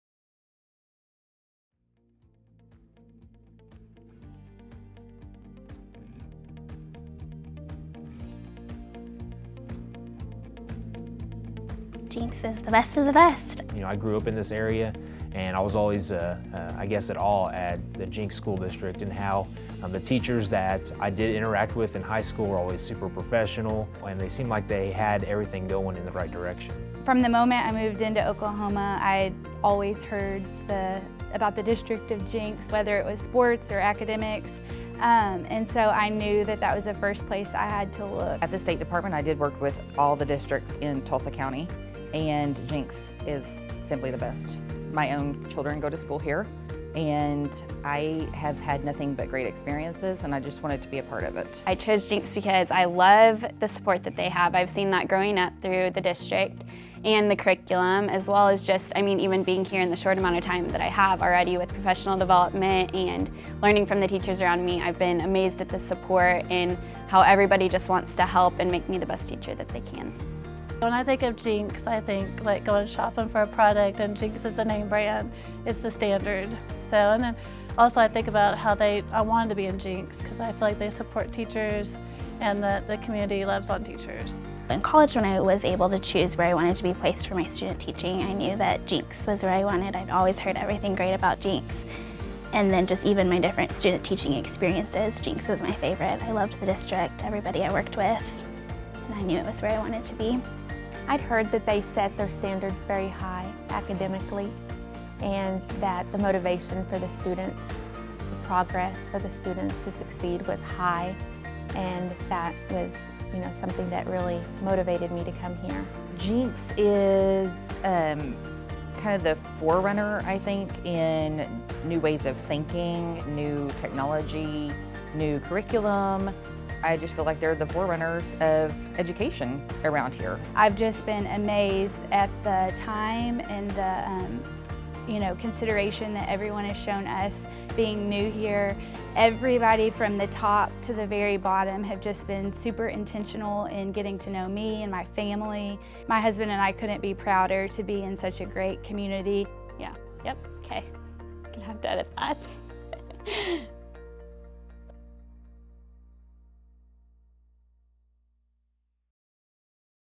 Why Do You Work at Jenks?
This year at JPS, we are asking our employees "what is your why?" Why are you driven to serve students? Why do you feel called to a career in public education? When you know your why, you work with a purpose. Watch some of our newest staff members explain why they are so proud to be part of the Trojan family.